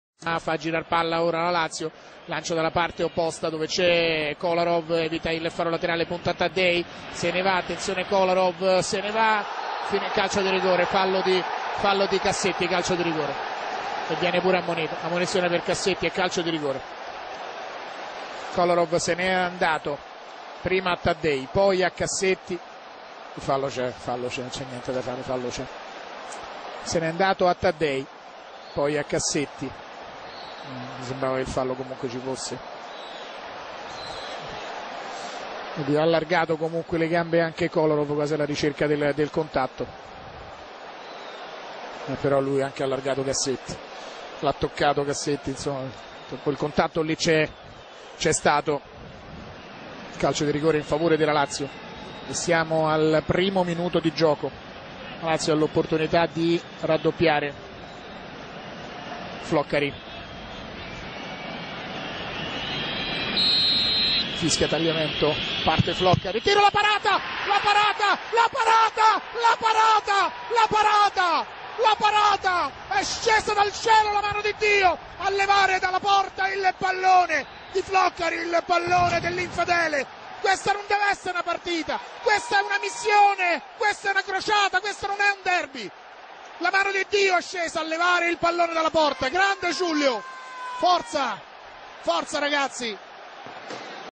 Lazio Roma 1 2 rigore parato Julio Sergio commento Zampa 18 4 2010.wmv
julio sergio santo subito